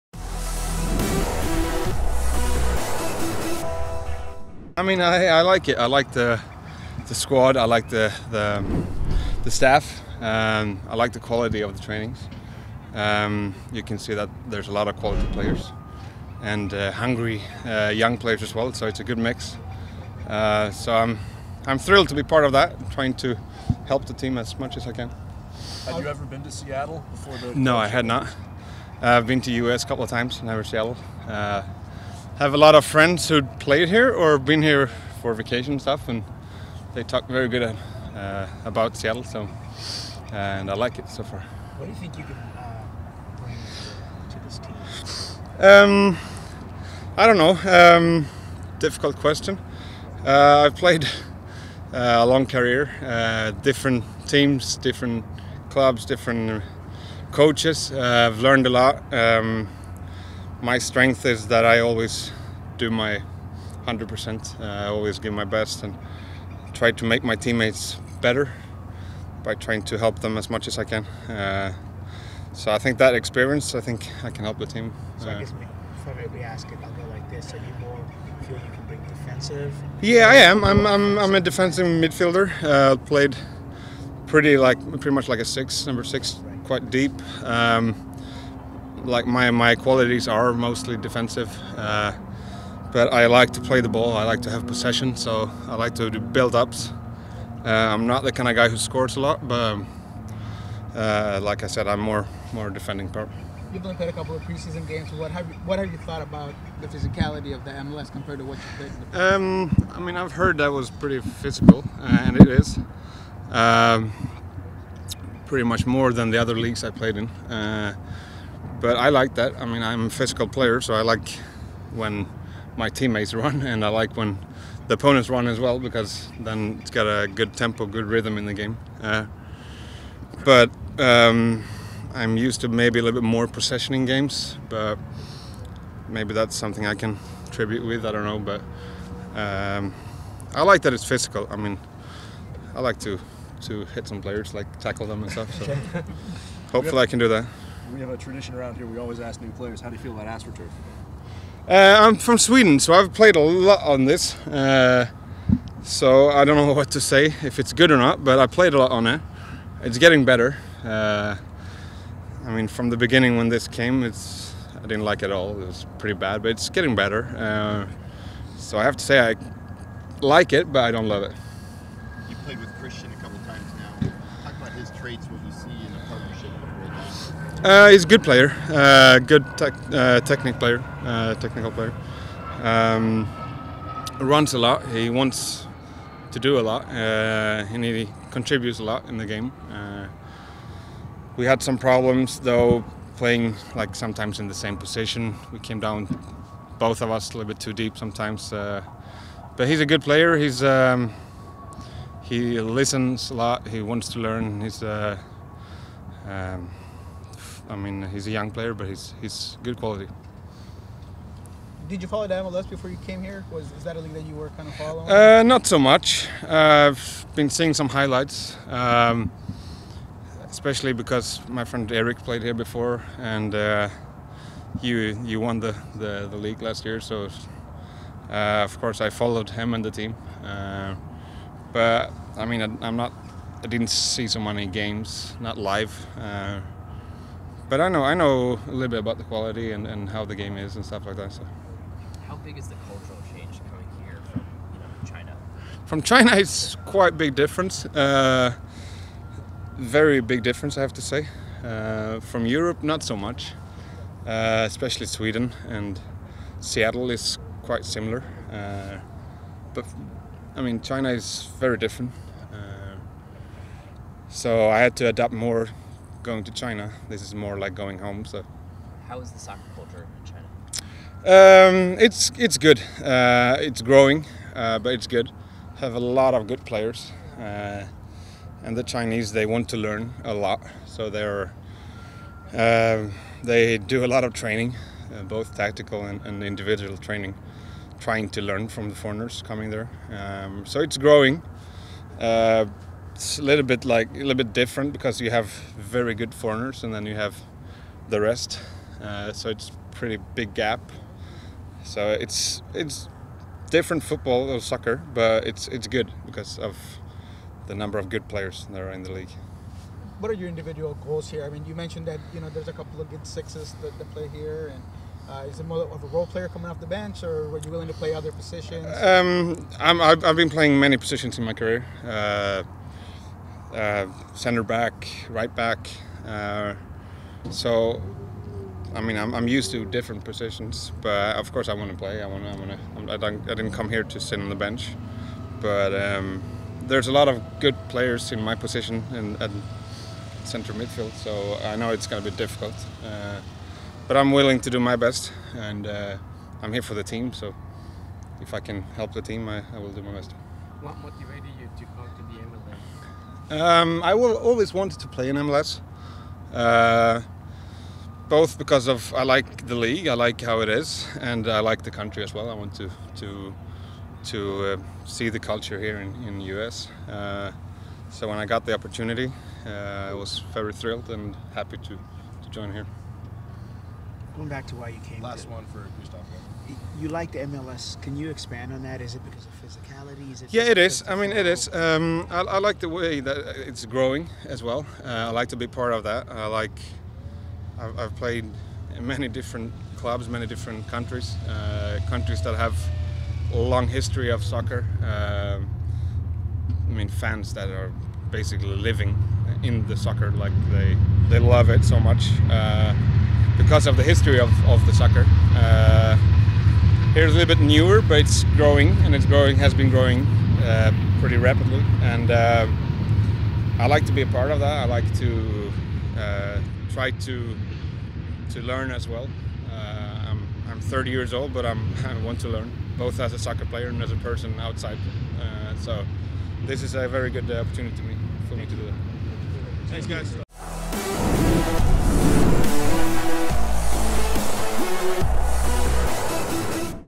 Interview: Gustav Svensson on the physicality of the MLS
Seattle Sounders FC midfielder Gustav Svensson speaks to media following training on the physicality of the MLS, on what he can bring to this club and on his chemistry with Cristian Roldan.